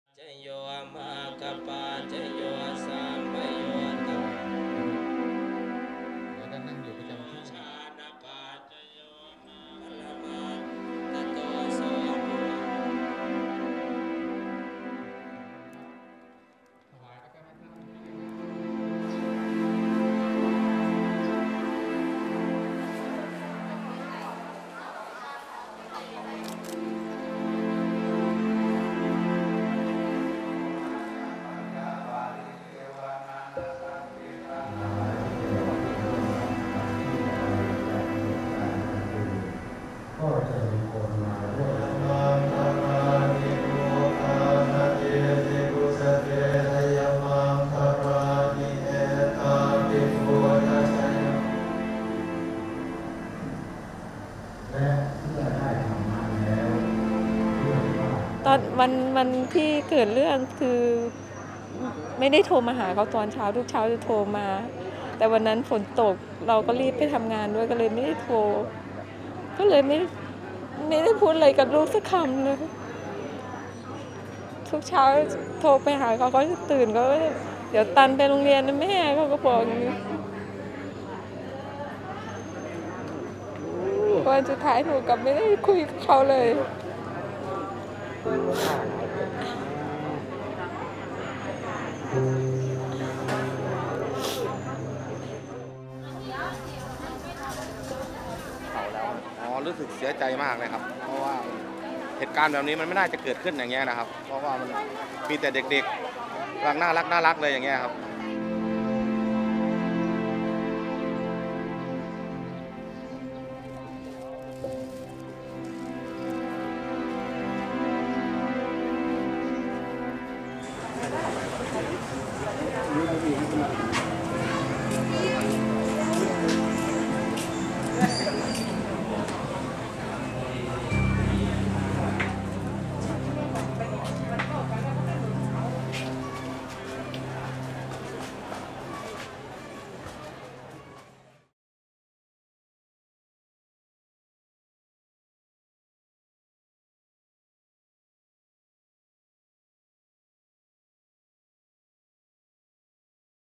Thailand holds mass cremation ceremony for victims of nursery mass killing
On October 11, 2022, Thai monks arranged a mass funeral and cremation ceremony at a Buddhist temple in Na Klang for 19 of the victims killed in a nursery school massacre. A former police officer murdered 36 people, including 24 children, on October 6 in one of the worst mass killings by a lone wolf attacker in Thailand’s recent history. Other victims will be cremated at other temples.